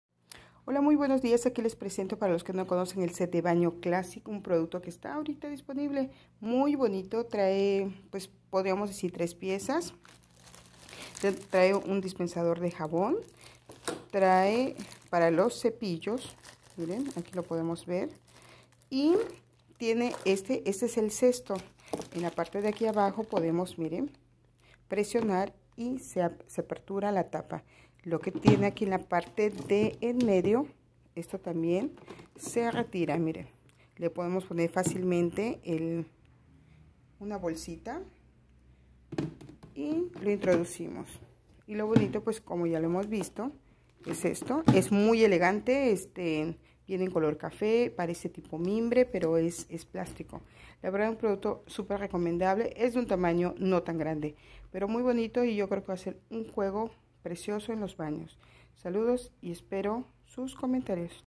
Set de Baño Clasic
Set de baño classic incluye 3 piezas material plástico en color chocolate tipo mimbre.